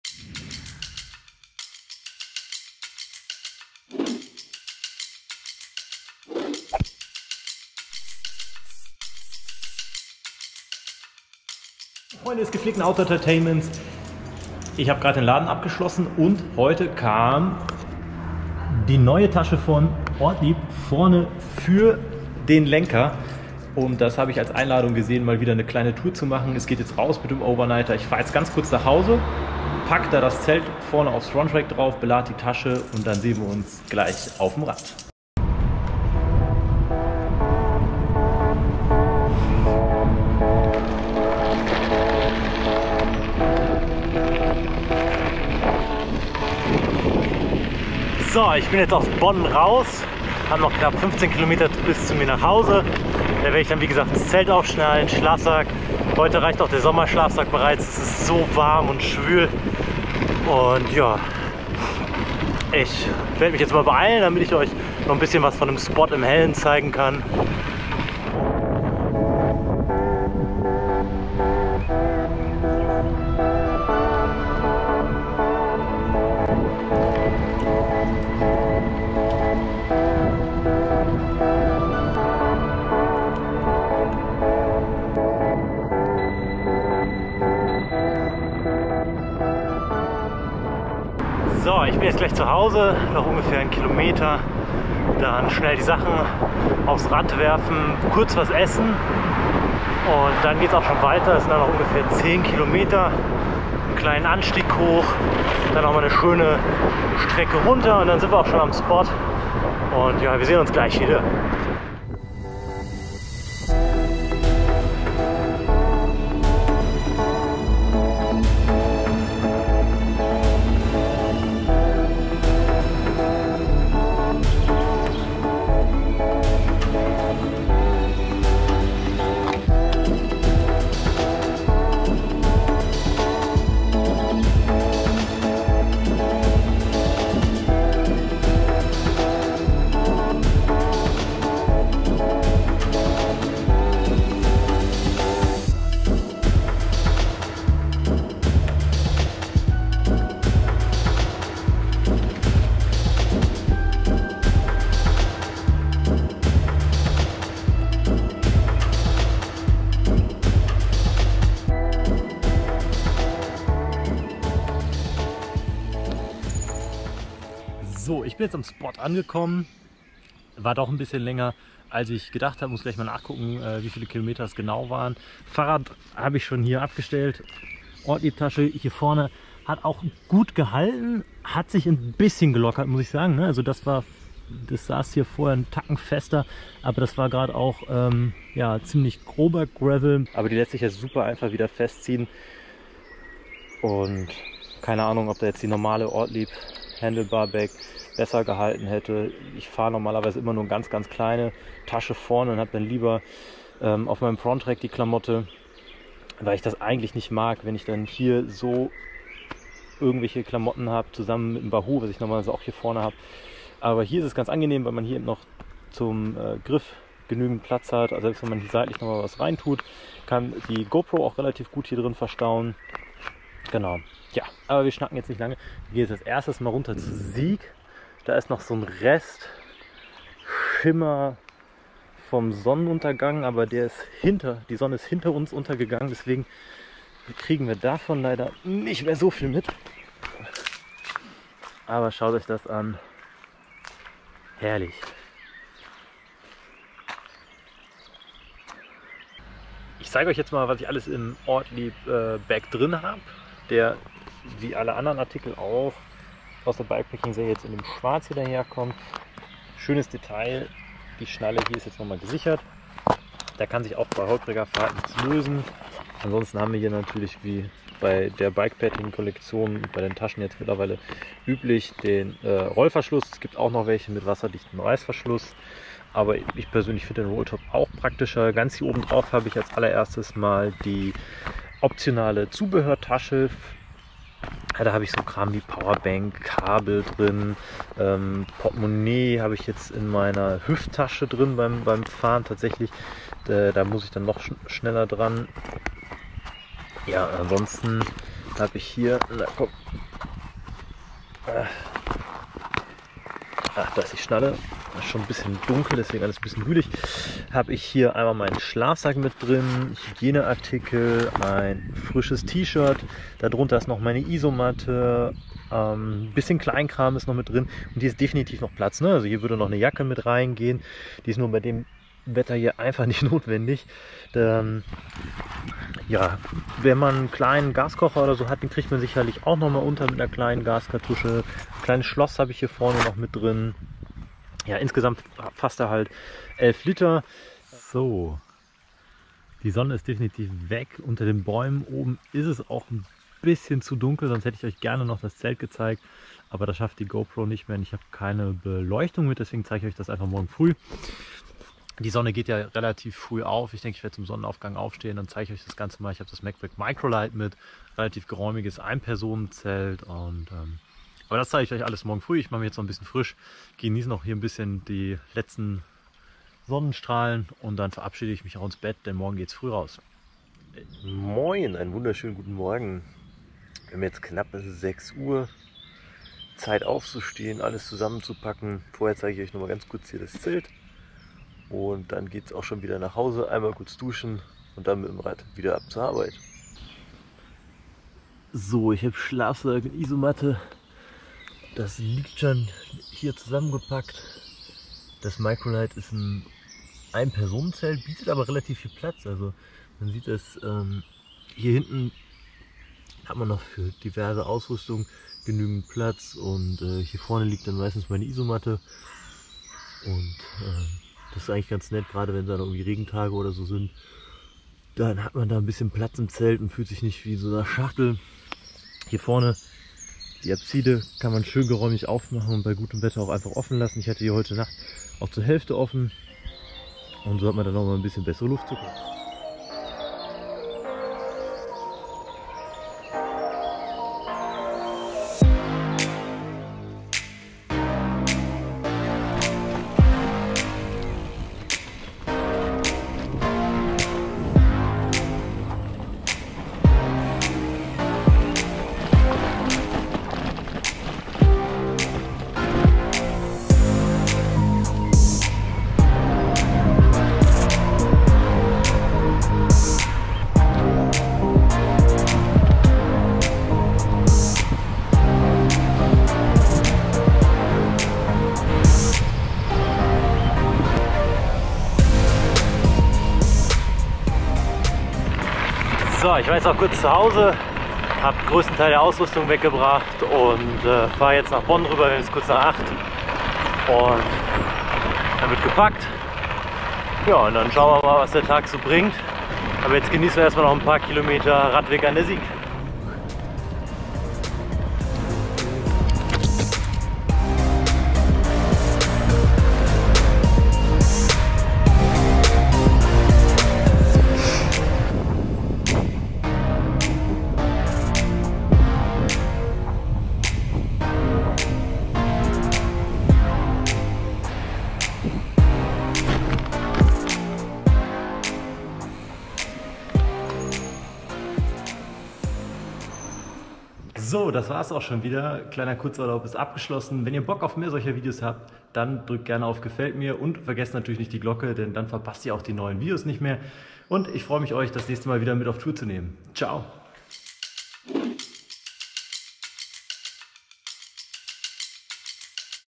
Bikepacking Tour mit dem neuem Ortlieb Handelbar Bag QR
Mojn Zusammen,

die Tour ist zwar schon ein paar Tage her, aber natürlich will ich sie euch nicht vorenthalten. Mit dabei war der neue Ortlieb Handelbarbag QR. Wie er sich geschlagen hat seht im Video. Besonders für kurze Overnighter Touren fahre ich ihn immer noch und bin begeistert vom Volumen. Bis auf mein Zelt passt wirklich alles rein.
Nachdem ich ihn nochmal neu eingestellt hat, verrutscht auch bei ruppigeren Abfahrten nichts mehr.

Habt ihr Bock auf mehr Videos dieser Art? Schreibt es mir gerne in die Kommentare!

Cheers
Jan